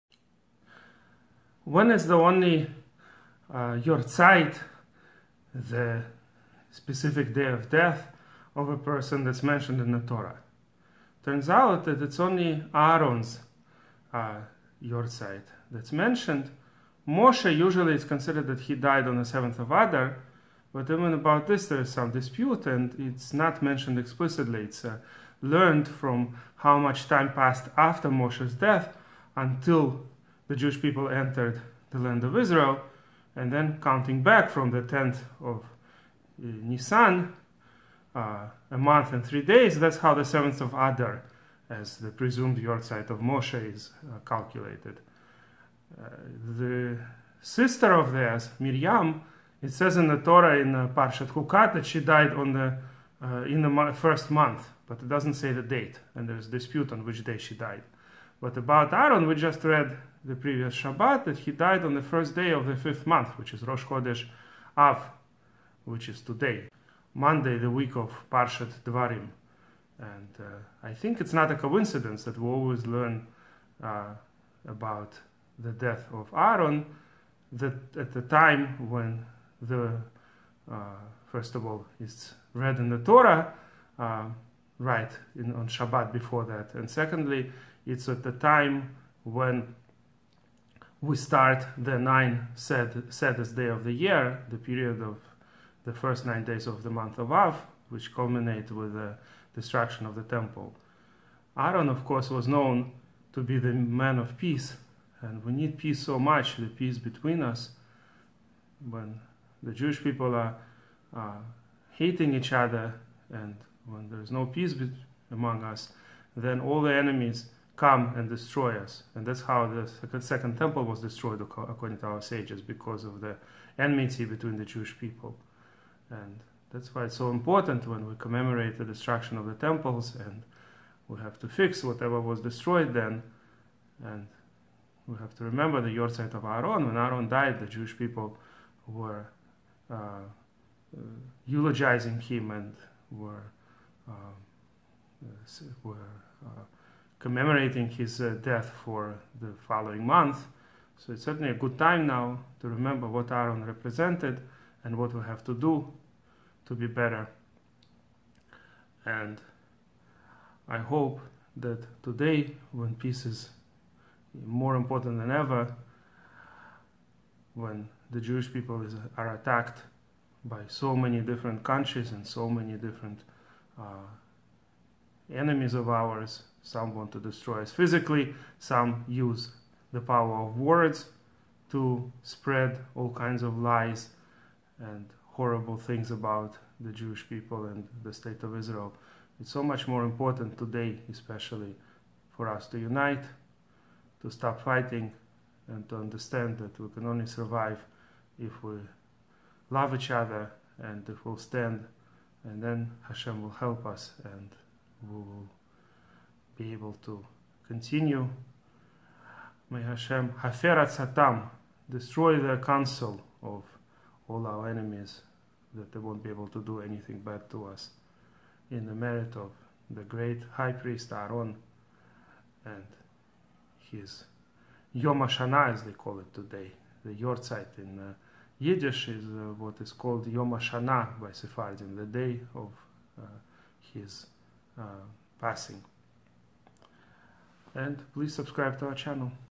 The only Yortzait that is mentioned in the Torah.
What we should learn from Aharon in the coming nine days.